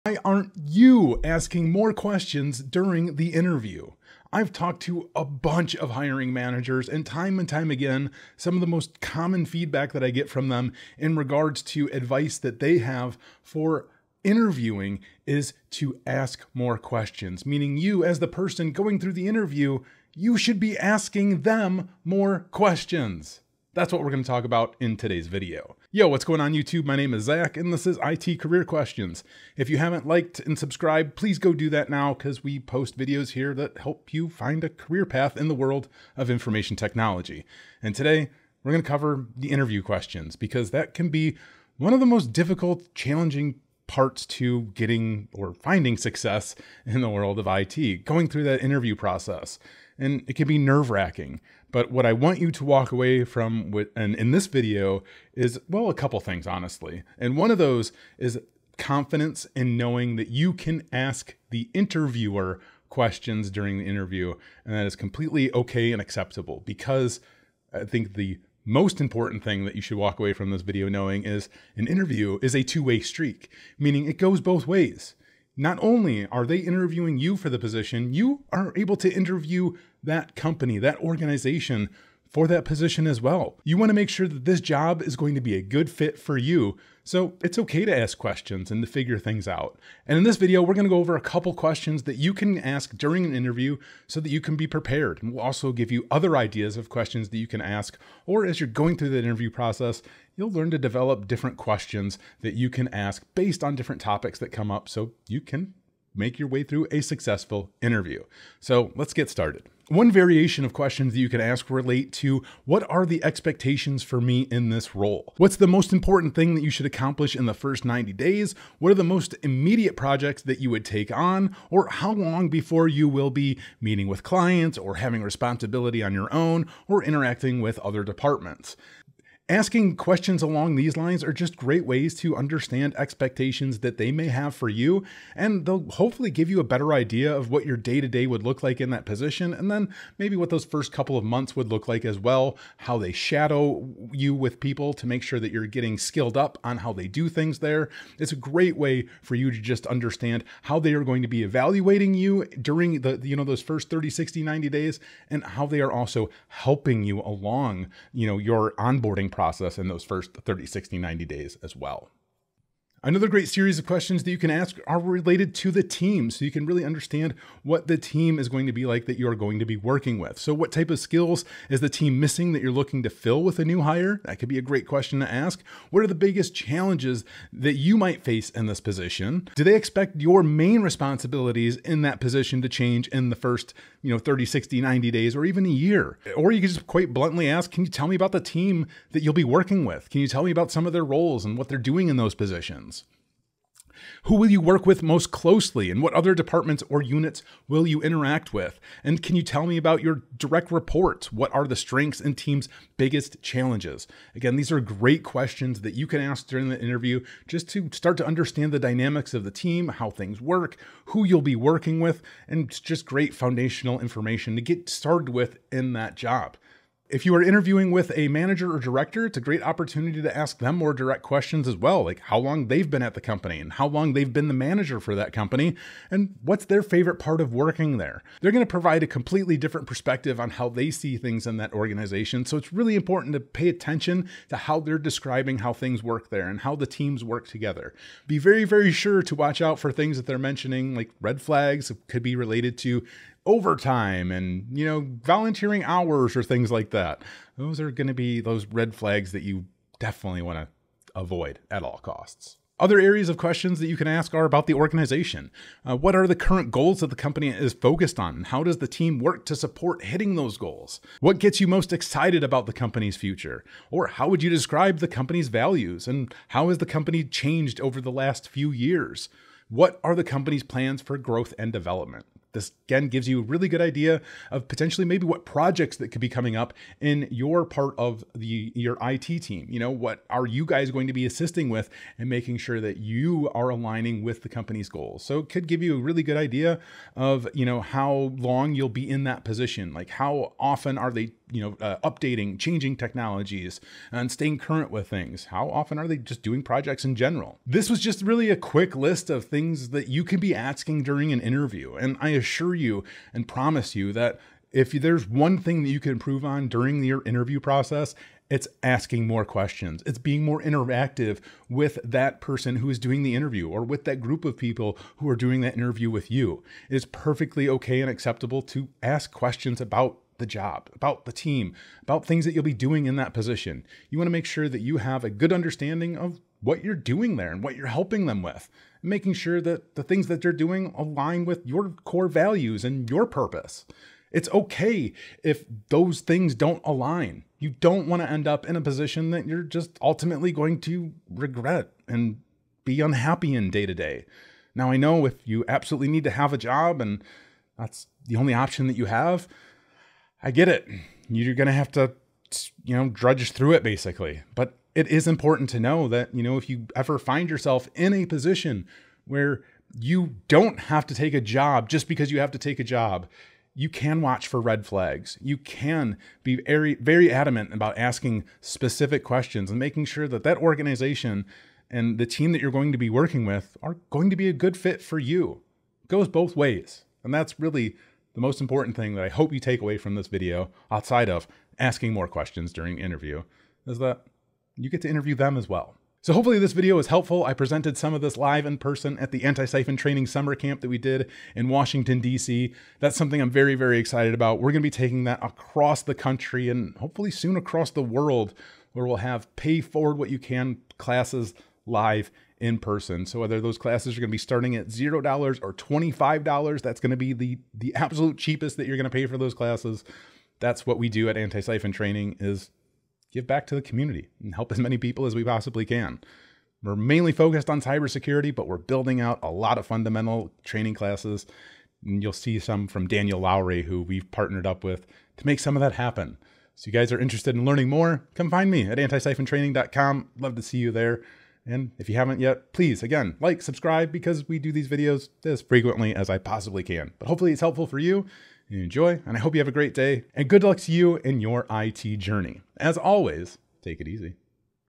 How to Ace Tech Job Interviews! Ask More Questions!
If you aren't asking questions during your job interviews, you're gonna have a bad time.

In this video, we provide a guide of interview questions that you should be asking during your job interviews.

Join me over at Antisyphon Training for free and affordable cybersecurity training

Thank you for visiting my YouTube channel.